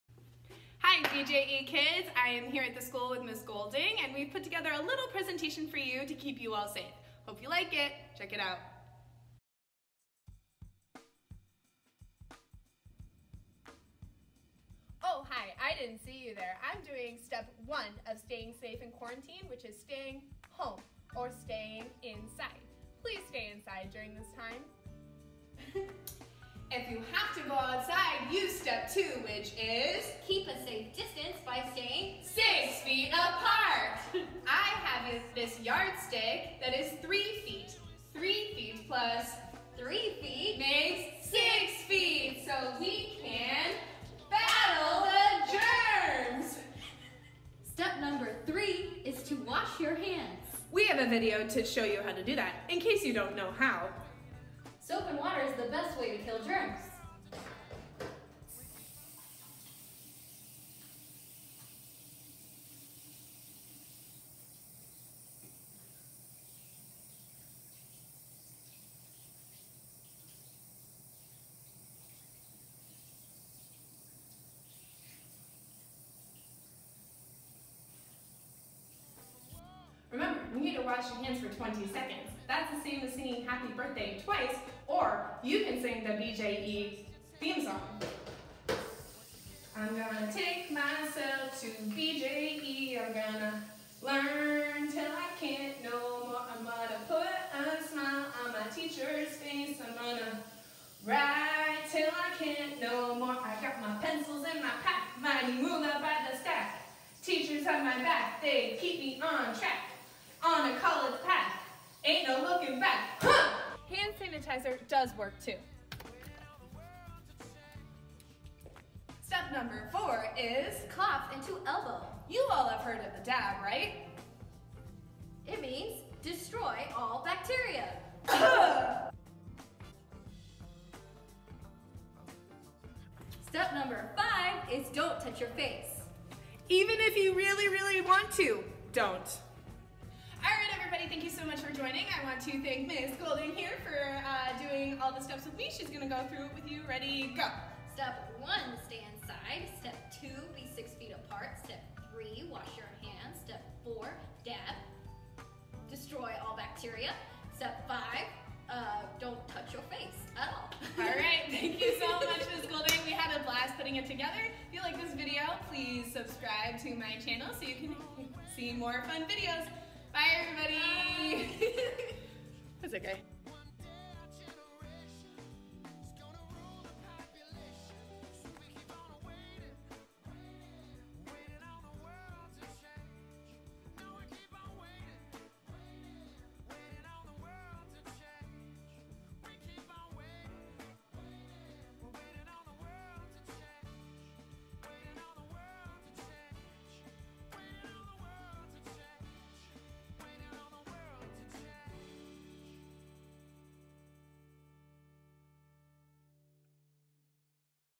Video on How to Stay Safe During Quarantine by Ms. Johnson and Ms. Golding
Watch as Ms. Johnson and Ms. Golding walk you through the 5 essential steps to stay safe during quarantine. Hope you like it and follow these steps!

We are following Google's 5 Finger Guidelines.